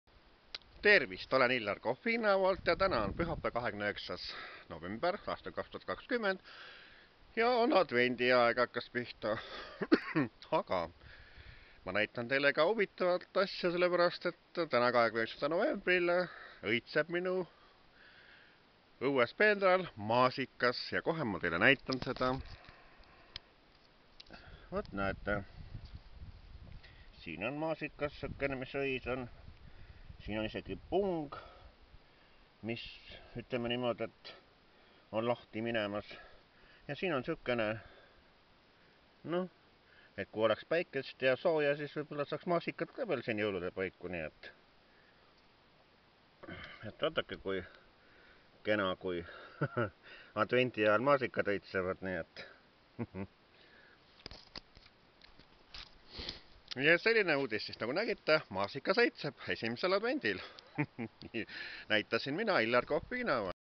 Hillar Kohvi õues peenral õitses 29. novembril ehk 1. advendil maasikas
Loodus on imeline ning valmistab üllatusi. 29. novembril 2020 ehk 1. advendil õitses minu õues peenral maasikas.